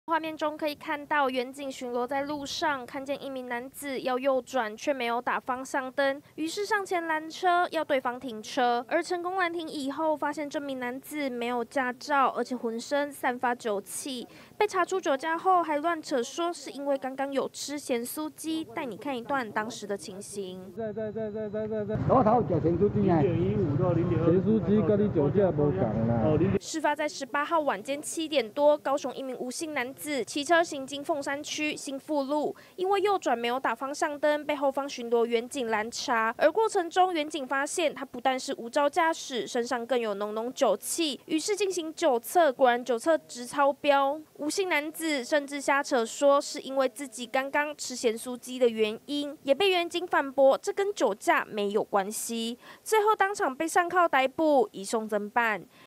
駕駛違規右轉又查酒駕 竟辯「吃鹹酥雞害的」【最新快訊】
高雄一名吳姓男子，騎車行經鳳山區，因為右轉，沒有打方向燈，被員警攔查，事後發現他違規酒駕，還扯說是因為吃鹹酥雞才闖禍，詳細情形連線記者 吳雪禎。